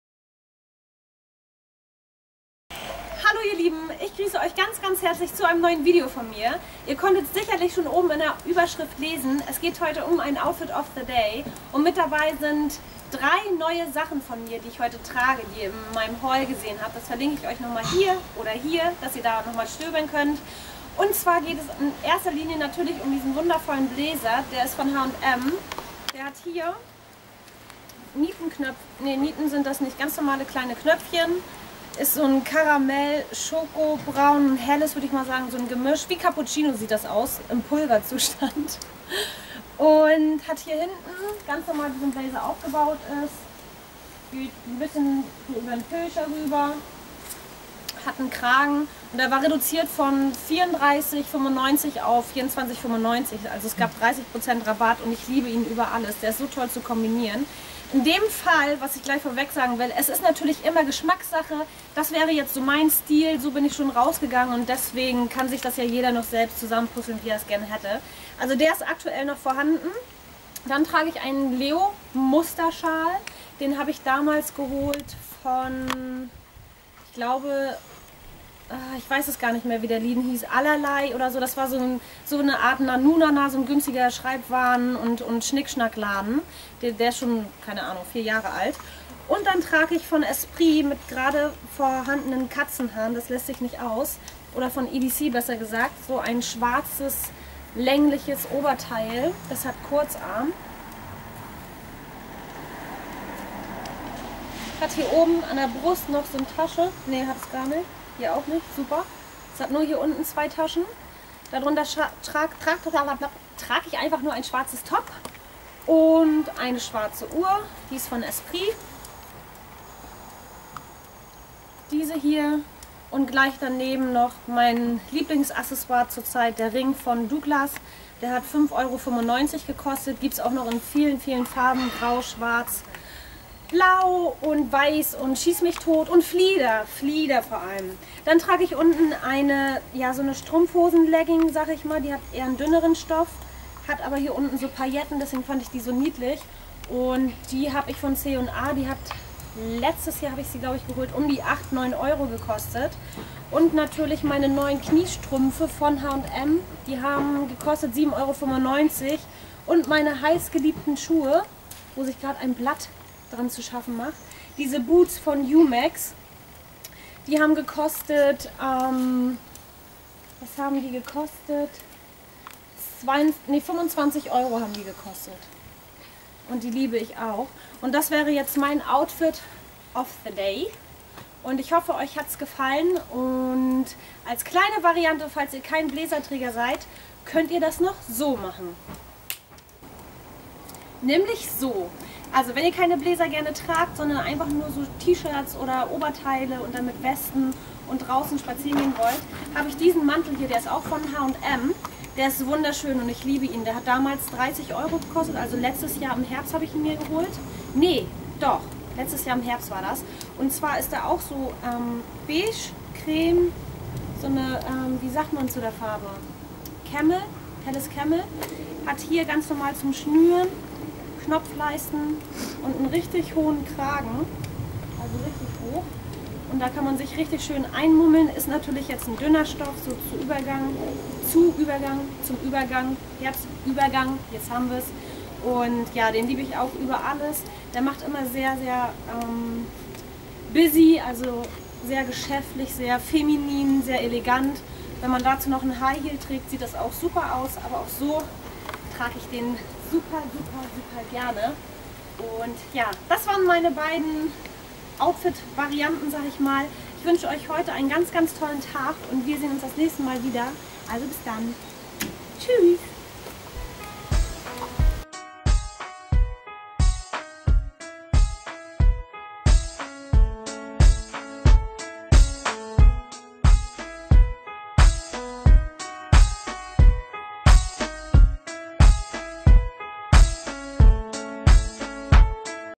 Outfit of the Day ♥
schön das ihr wieder eingeschaltet habt ♥

Heute zeige ich eins meiner Outfit wie ich es getragen habe. Herbstlich angehaucht und megaaaa gemütlich, ich hoffe es gefällt Euch:)

Natürlich sind Outfits Geschmackssache und Geschmäcker sind ja unterschiedlich, deswegen dient es zur Inspiration, denn jeder kann ja Kleidungsstücke ersetzen oder anders kombinieren ;)

Wünsche euch gaaanz viel Spass und einen schönen Sonntag ♥ hoffe das wackeln stört nicht allzu sehr *hihi*

Kussi eure
Jule :-*

Auf den Nägeln trage ich:
YSL - No. 36
Essence - 01 Wake me up for Vegas

Würde mich freuen wenn wir uff

oder neuerdings

schnacken ♥